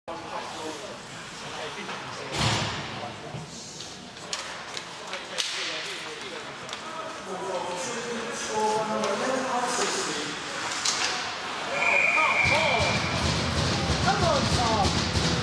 20190202 Lee Valley Lions vs Swindon Wildcats 0021 GA
20190202 Lee Valley Lions vs Swindon Wildcats
Lee Valley Lions vs Swindon Wildcats NIHL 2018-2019 Ice Hockey League Game London England Canon Legria HF S21
for more ice hockey videos)